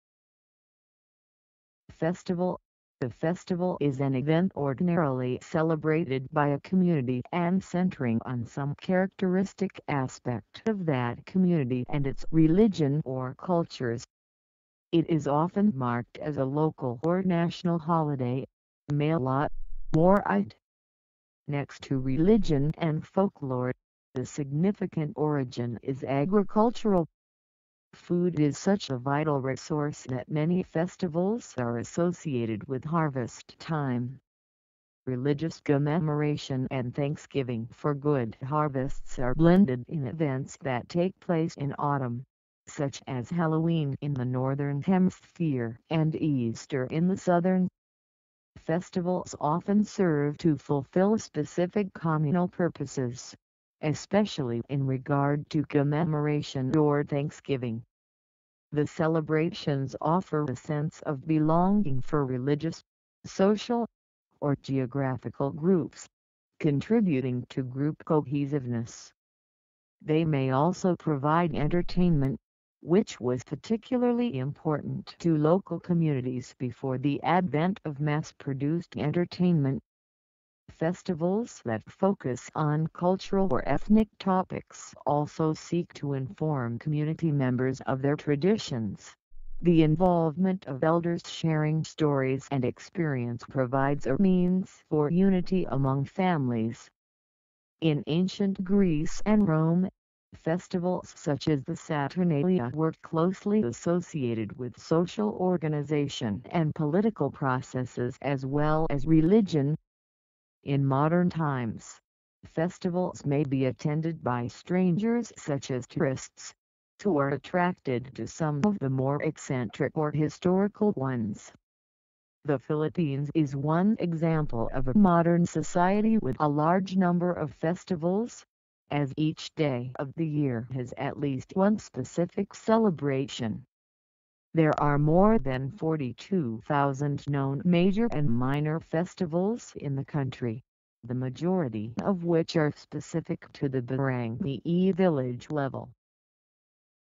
What Is FESTIVAL? FESTIVAL Definition & Meaning
What is FESTIVAL, What does FESTIVAL mean, FESTIVAL meaning, FESTIVAL definition, FESTIVAL explanation

A festival is an event ordinarily celebrated by a community and centering on some characteristic aspect of that community and its religion or cultures. It is often marked as a local or national holiday, mela, or eid. Next to religion and folklore, a significant origin is agricultural. Food is such a vital resource that many festivals are associated with harvest time. Religious commemoration and thanksgiving for good harvests are blended in events that take place in autumn, such as Halloween in the northern hemisphere and Easter in the southern.

Festivals often serve to fulfill specific communal purposes, especially in regard to commemoration or thanksgiving. The celebrations offer a sense of belonging for religious, social, or geographical groups, contributing to group cohesiveness. They may also provide entertainment, which was particularly important to local communities before the advent of mass-produced entertainment. Festivals that focus on cultural or ethnic topics also seek to inform community members of their traditions; the involvement of elders sharing stories and experience provides a means for unity among families.

In Ancient Greece and Rome, festivals such as the Saturnalia were closely associated with social organisation and political processes as well as religion.[1][2][3] In modern times, festivals may be attended by strangers such as tourists, who are attracted to some of the more eccentric or historical ones. The Philippines is one example of a modern society with a large number of festivals, as each day of the year has at least one specific celebration. There are more than 42,000 known major and minor festivals in the country, the majority of which are specific to the barangay (village) level.[4]
Source: Wikipedia.org